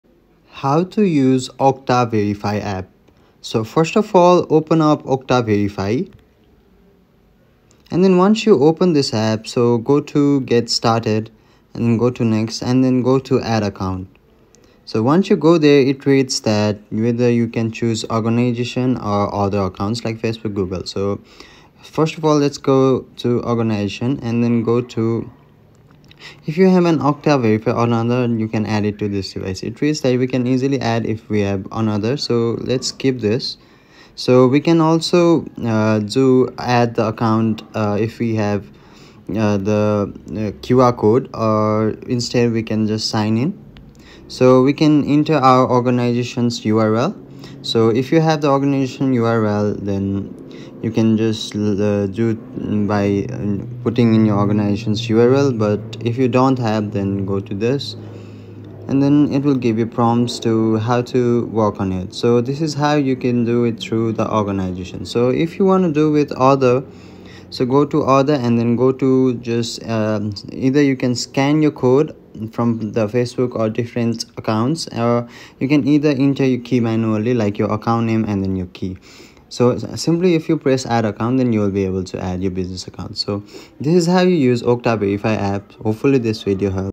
How to Use Okta Verify App (Step-by-Step Setup & Login Guide)
New to Okta Verify? This step-by-step guide shows you exactly how to use the Okta Verify app — whether you’re adding an organization account or setting up multi-factor authentication for personal logins like Google or Facebook.

What you’ll learn:
 • How to add an organization or other account
 • How to scan QR codes or sign in manually
 • Understanding account types and keys
 • Using Okta Verify for secure login
 • Accessing settings and managing preferences

Perfect for students, employees, or anyone setting up Okta 2FA for the first time.